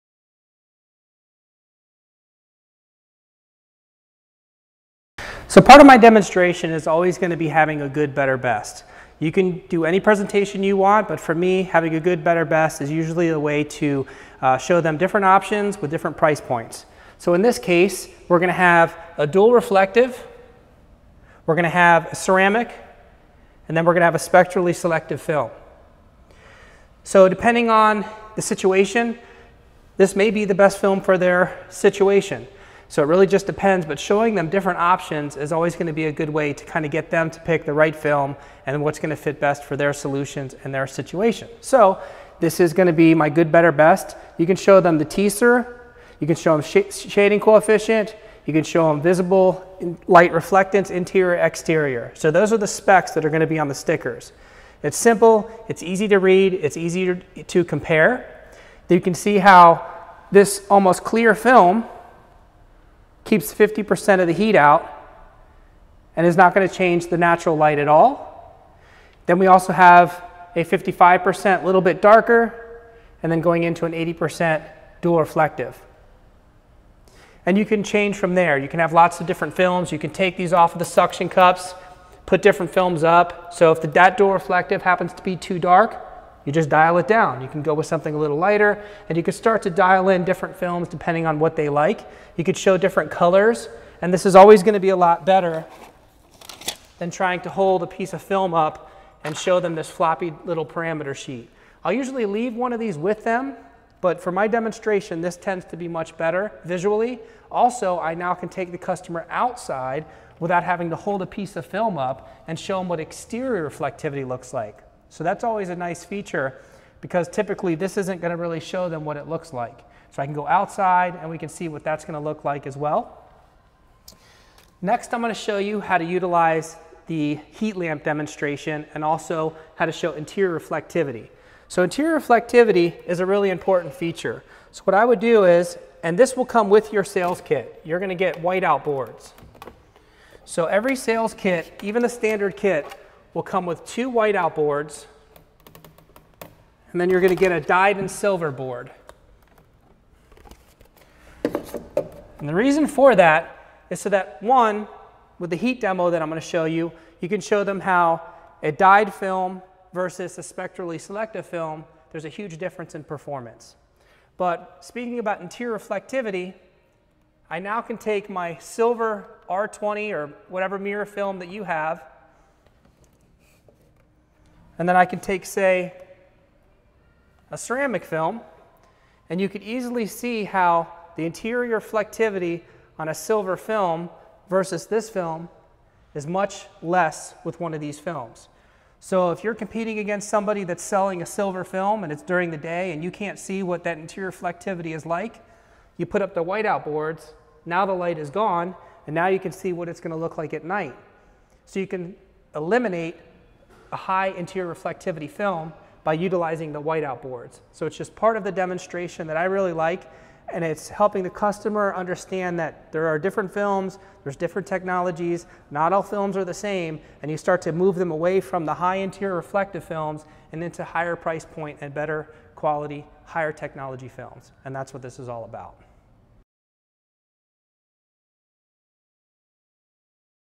Window Tinting Sales
We demonstrate how different films will perform based on technology. You don't have to go with a dark or reflective film to keep out the heat... check it out!